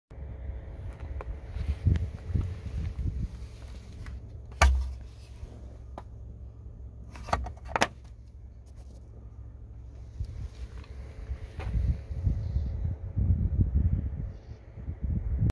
2018 To 2023 Kia Rio How To Fill Windshield Washer Fluid - Filler Cap & Tube Location
Windshield Washer Fluid
Washer Fluid Concentrate
Washer Fluid Tablets
Winter De-Icer Washer Fluid

This quick DIY video shows owners of the 4th generation YB 2018, 2019, 2020, 2021, 2022 and 2023 Kia Rio how to locate the filler cap for the windshield washer fluid reservoir bottle.